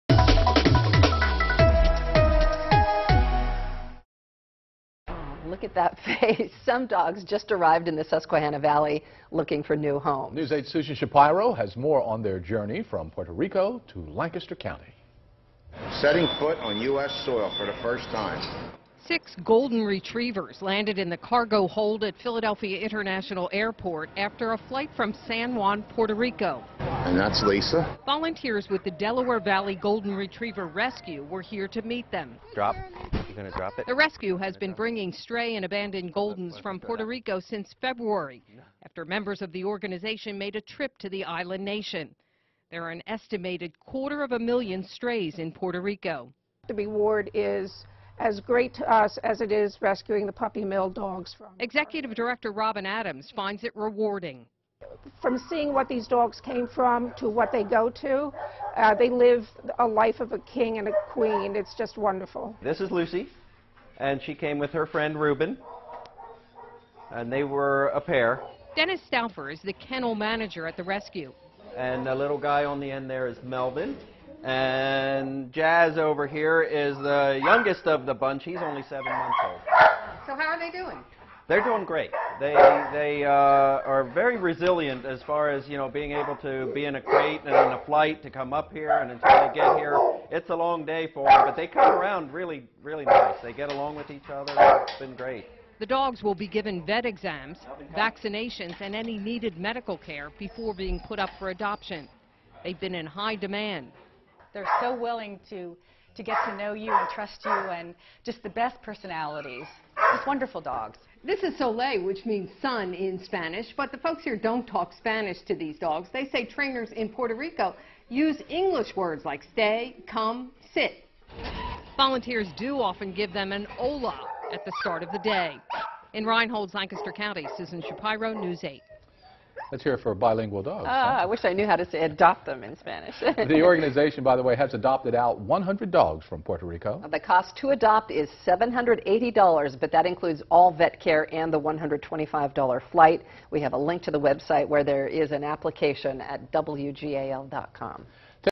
Golden retrievers looking for new homes
Six golden retrievers just arrived in the Susquehanna Valley and are looking for new homes.